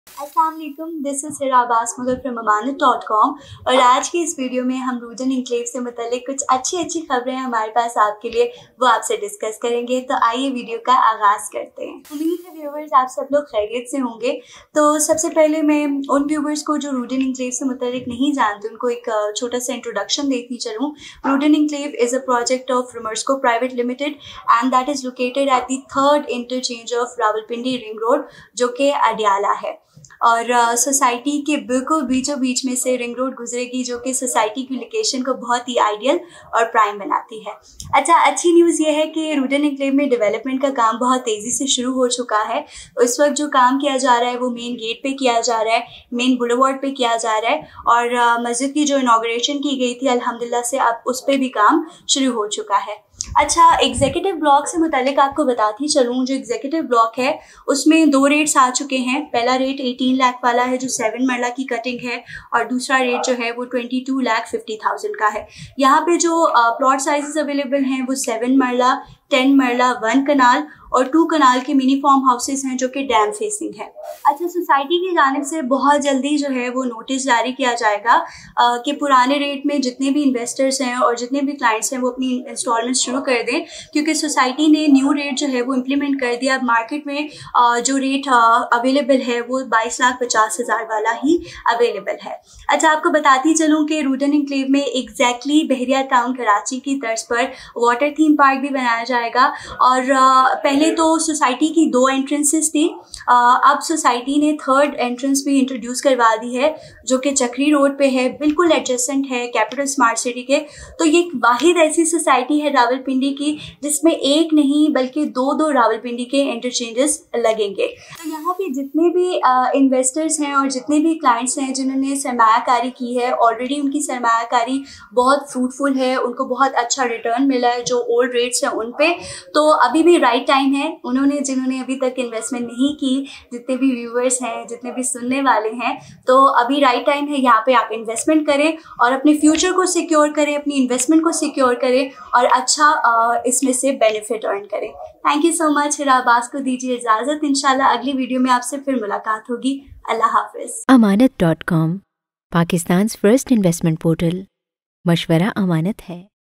RUDN Envclave Latest News | Development updates of RUDN Enclave | Ammanat.com
RUDN is a very well developed residential society from where Rawalpindi and Islamabad can easily be reached it is developed for the low income citizen who want their dream house. RUDN wants to fulfill this dream by providing them with the most luxurious and sustainable society.
One of the main feature of RUDN Enclave is its executive location. Everybody wants to live at an ideal place. Rawalpindi is a very crowded city which a lot of traffic jams but the location of RUDN Enclave is present at a very ideal location.